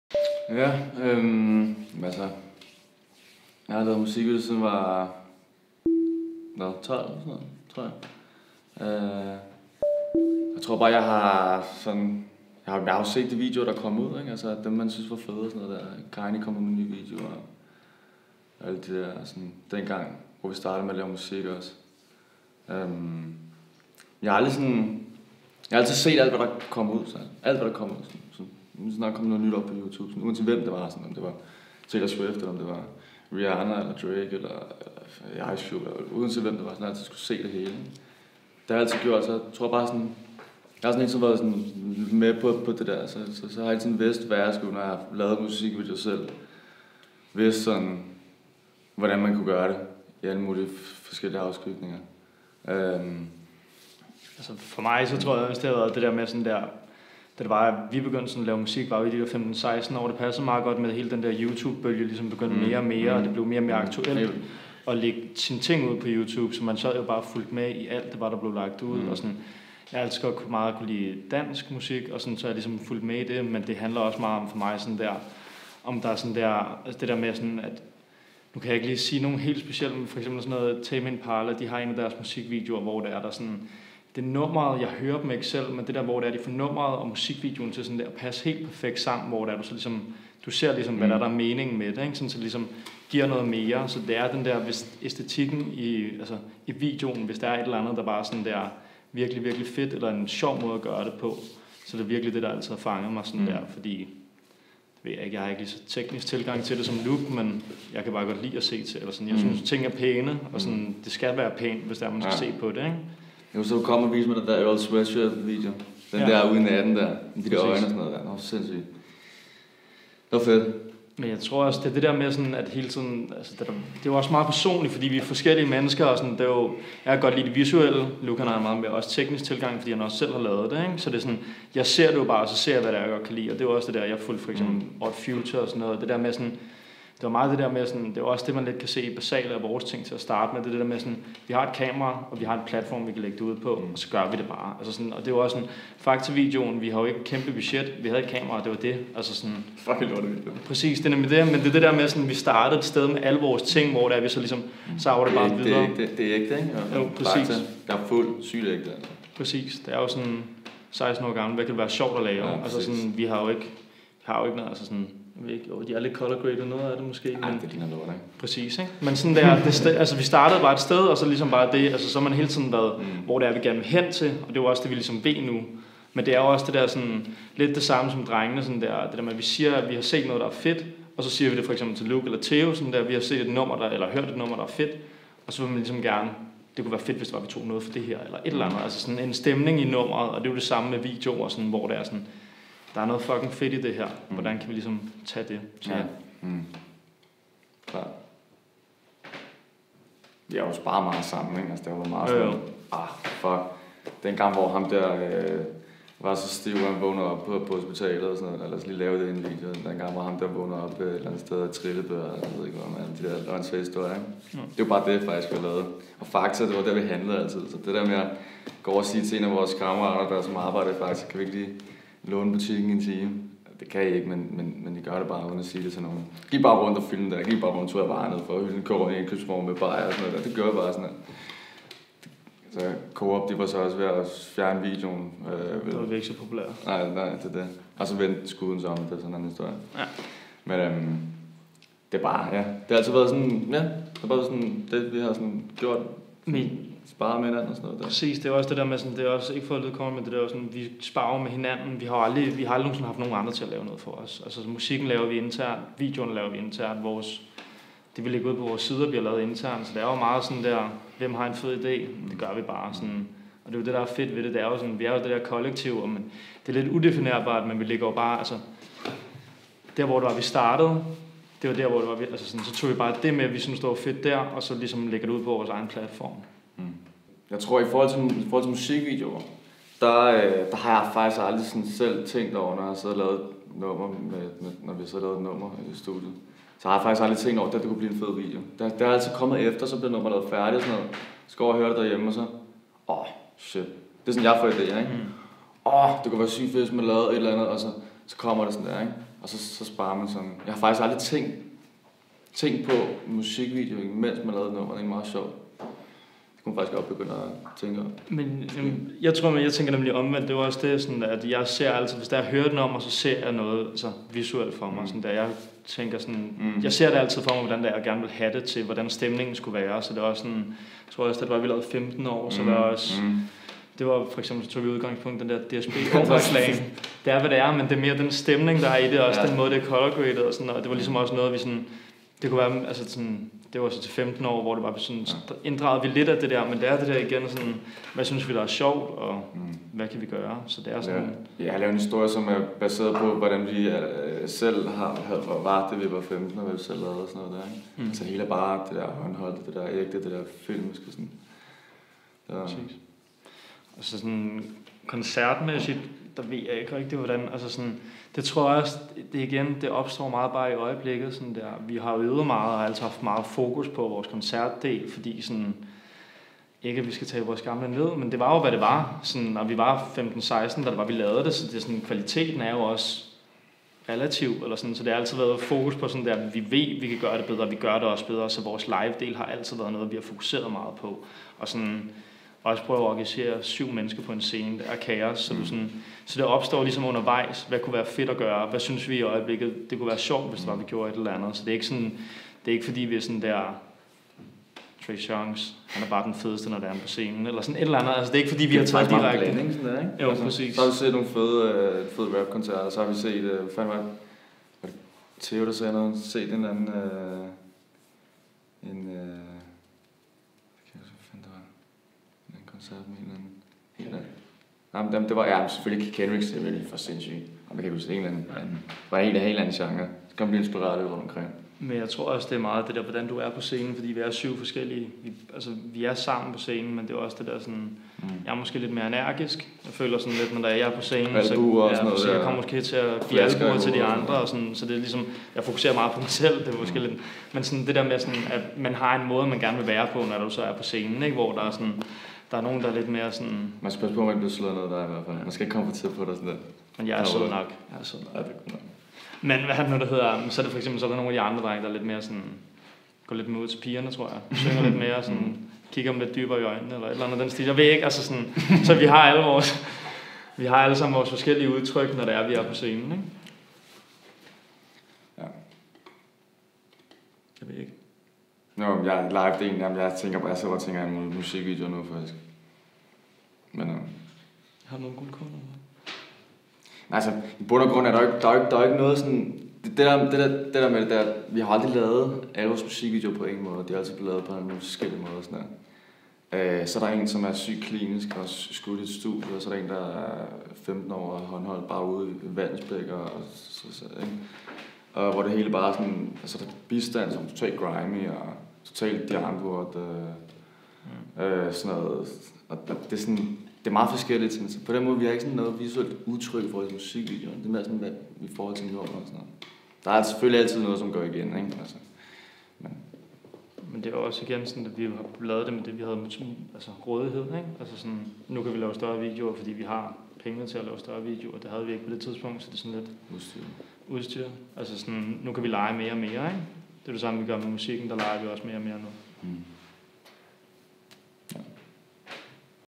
DE DANSKE HYRDER talks Visual Identity + Odd Future, Youtube and Improvements with WHAT ERA
Production By: Jonas Risvig, Frederik Borg, Asbjørn Dam and Frederik Pedersen